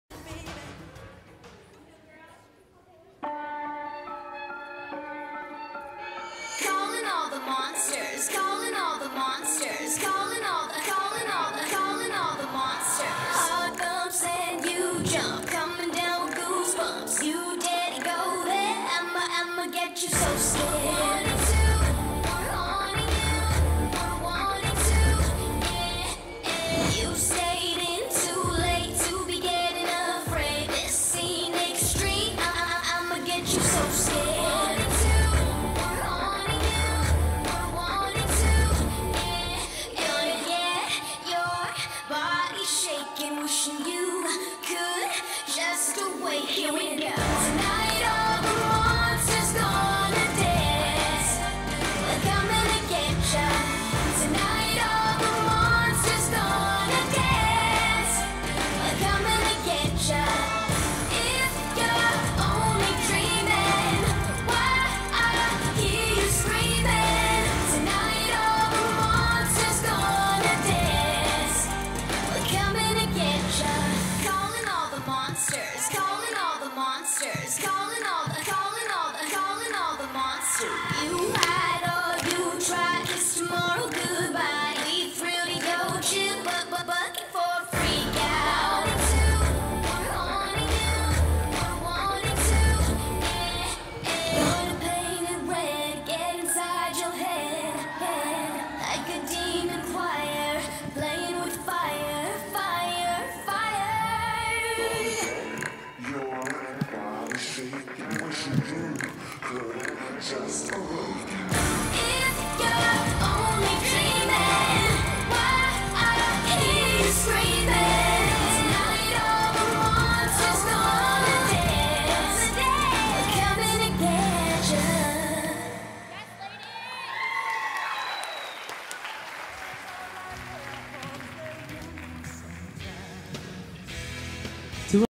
Carolina Collective Dance - They're Haunting You
Junior • Small Group • American • Jazz • Age 9
CAROLINA COLLECTIVE DANCE
HOF Jacksonville 2022